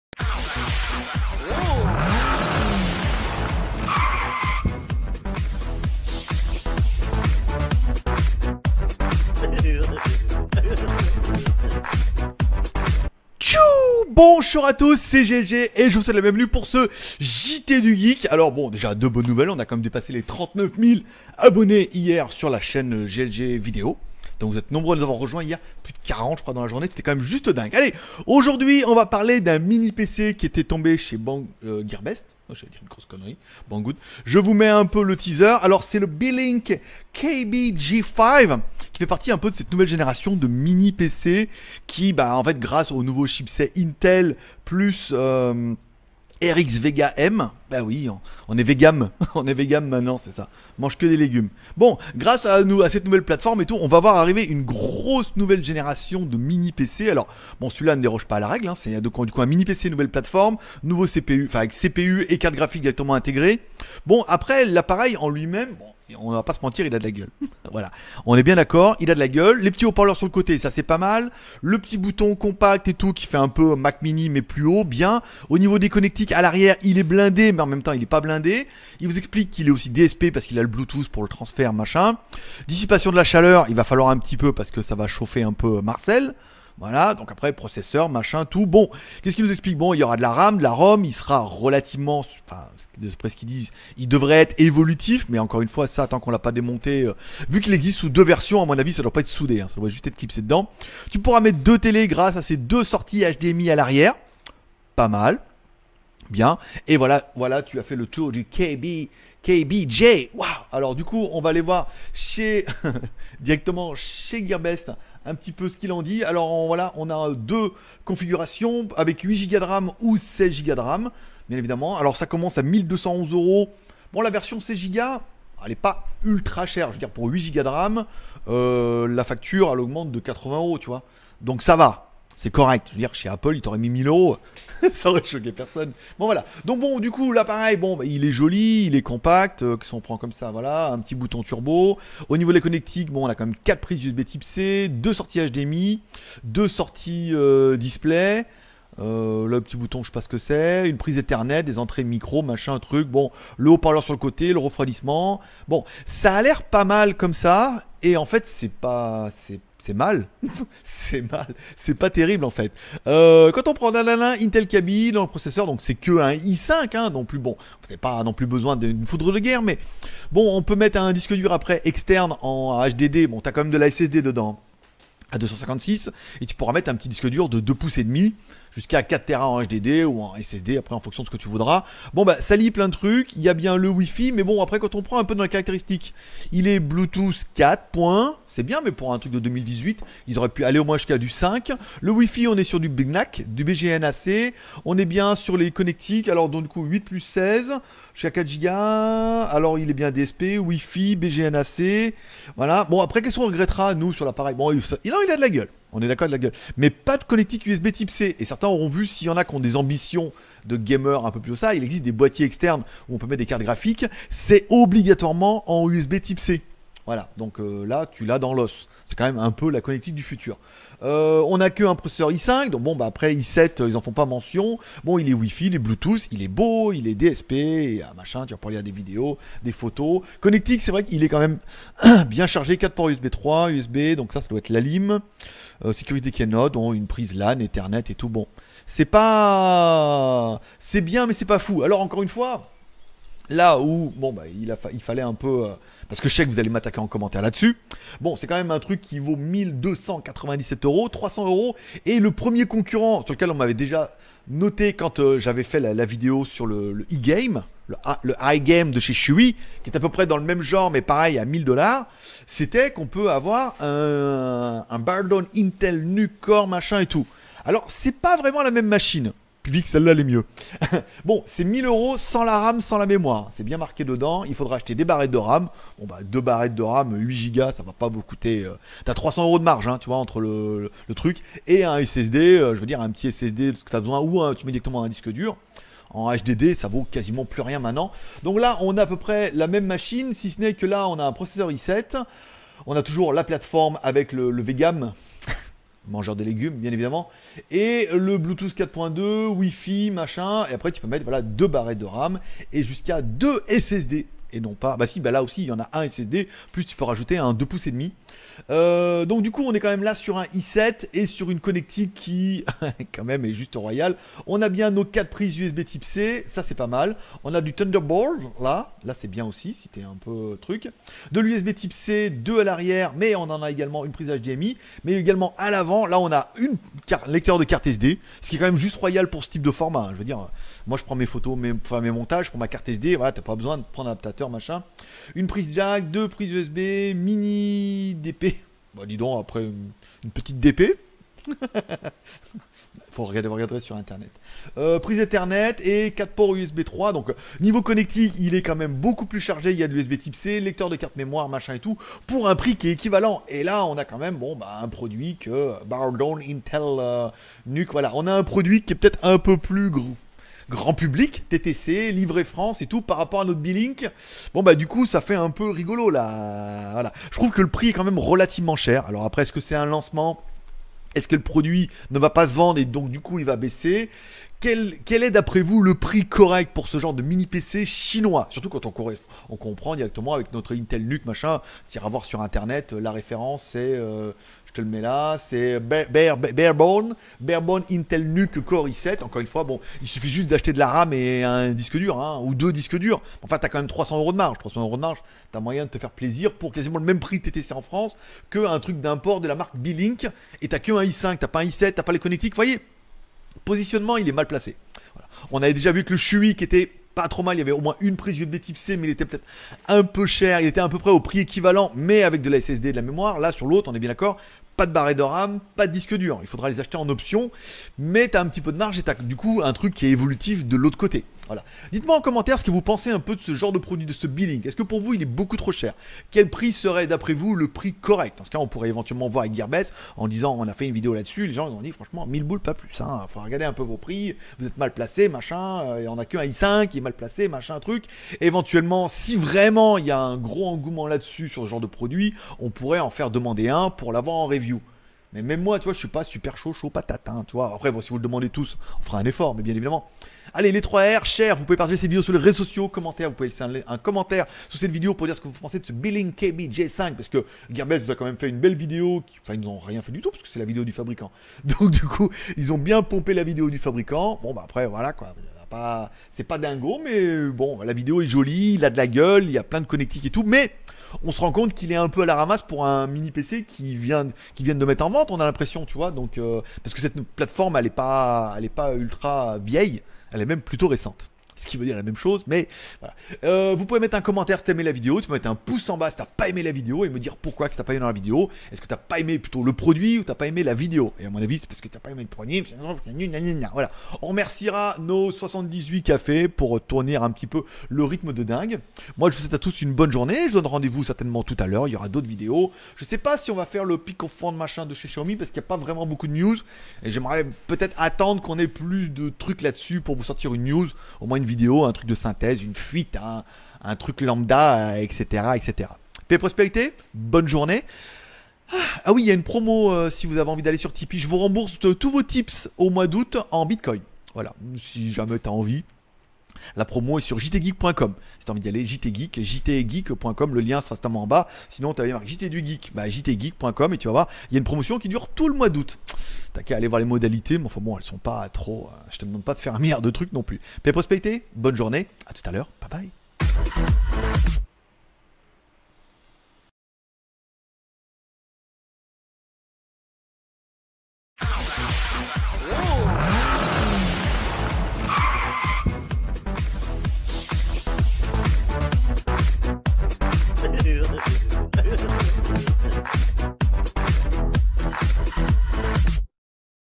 Mini Pc Chinois Beelink Kaby G5 i5 à 1200€ ! JTG par GLG
La nouvelle plateforme Intel + RX Vega M va donner naissance à une nouvelle génération de Mini pc comme ce Beelink Kaby G5 avec processeur I5, 8/16Gb ram + 256Gb en SSD avec un prix d'attaque à 1211€, décryptage obligatoire !